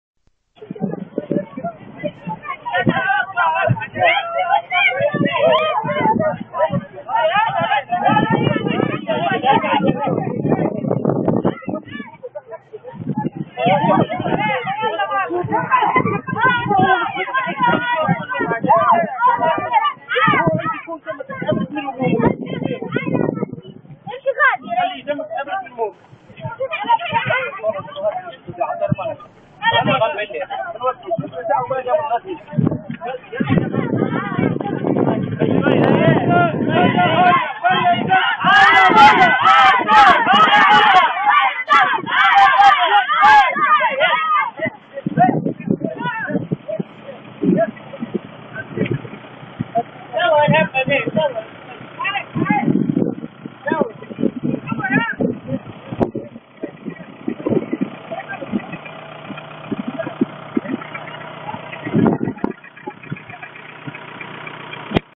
Manifestation à la décharge Jbel Borj Chakir. Dimanche 23 mars 2014
Manifestation des résidents des zones avoisinantes de la décharge de déchets Jbel Borj Chakir. Dimanche 23 mars 2014. Reportage aux nouvelles de 20 h sur Watanya 1